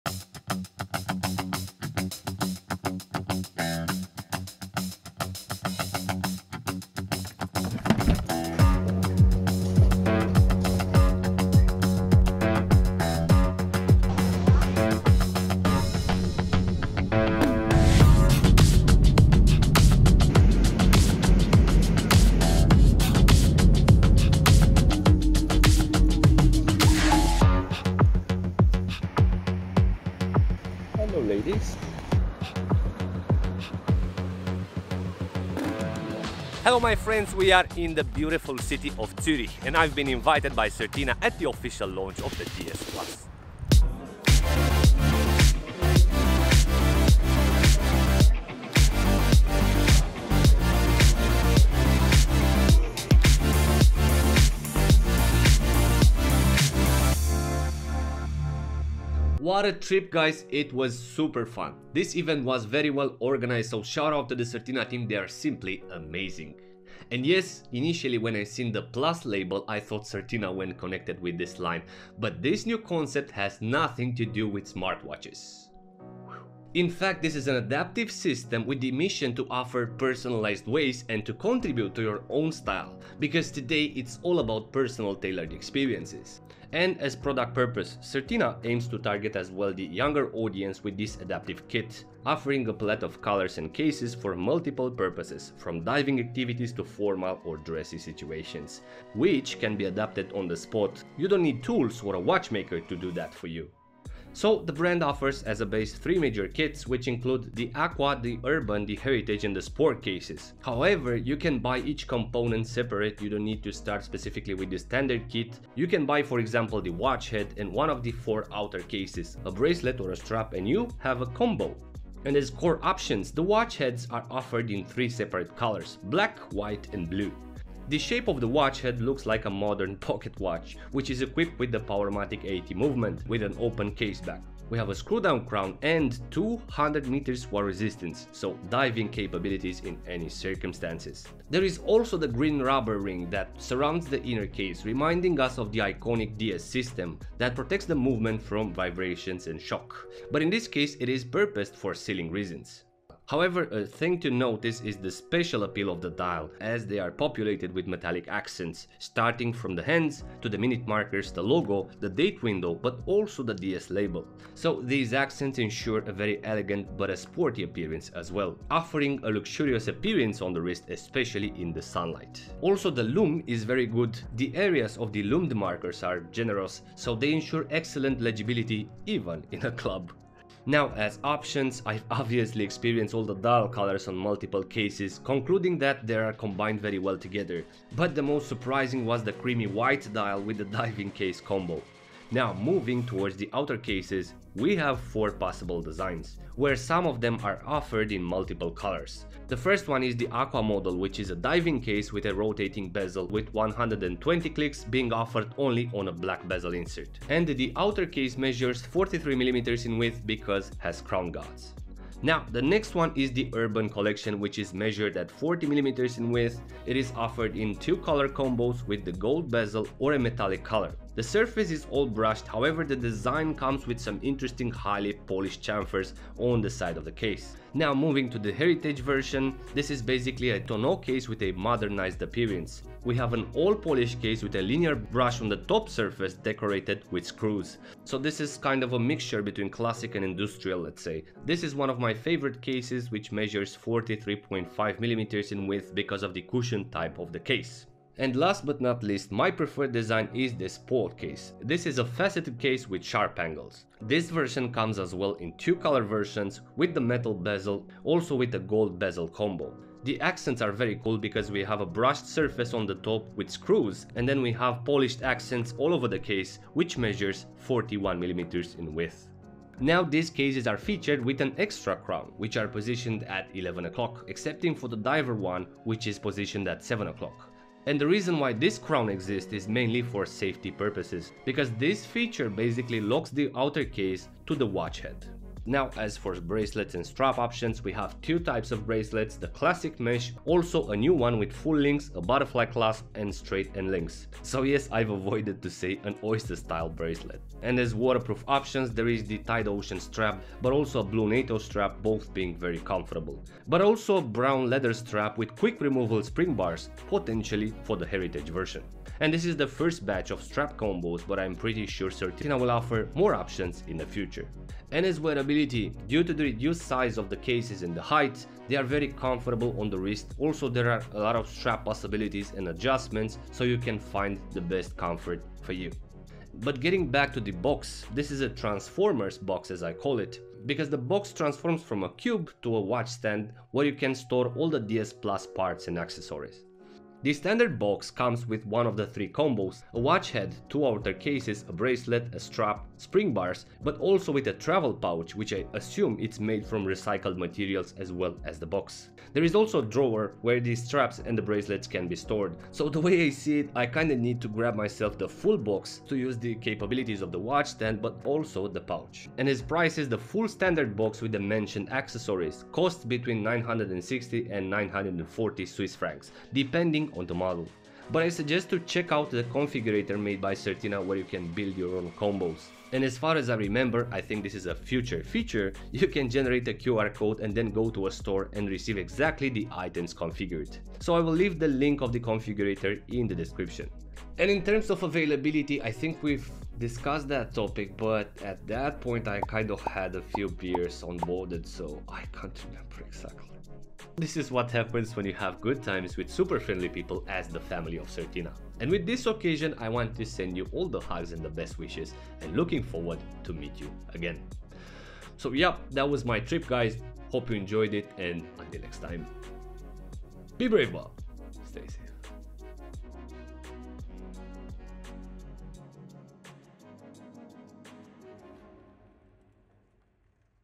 All you need to know about the new Certina DS+ concept! directly from the event ;)
Hello my friends & Certina enthusiasts.
This week I had the pleasure to attend to the official launch of the new Certina DS+ in Zurich, so I made a first impressions video about this adaptive concept, Enjoy!